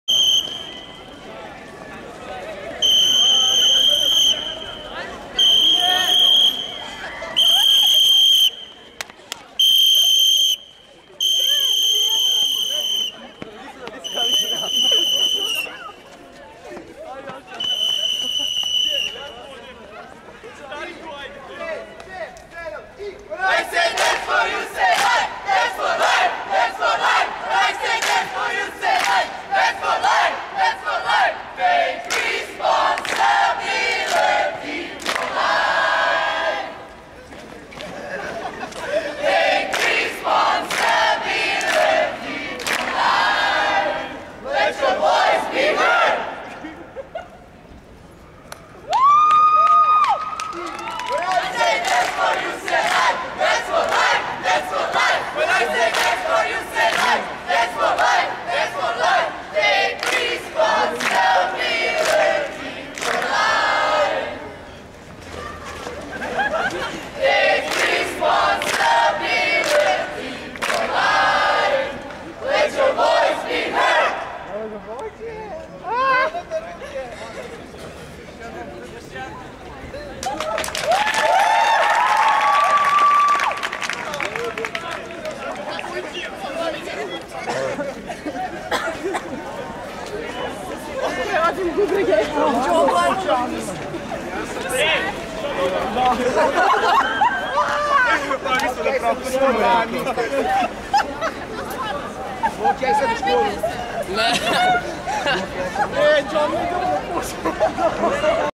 Dance for life 2011 - Novi Sad igrom protiv side
Mladi iz novosadskih plesnih klubova pridružili se manifestaciji Dance 4 life koja ima za cilj da upozori mlade na opasnosti koje nosi SIDA.
    Projekat Dance4Life nastao je kao direktan odgovor na činjenicu da HIV i AIDS najviše pogađaju mladu populaciju. Od 12.000 ljudi koji obole svakog dana, više od polovine čine mlađi od 25 godina.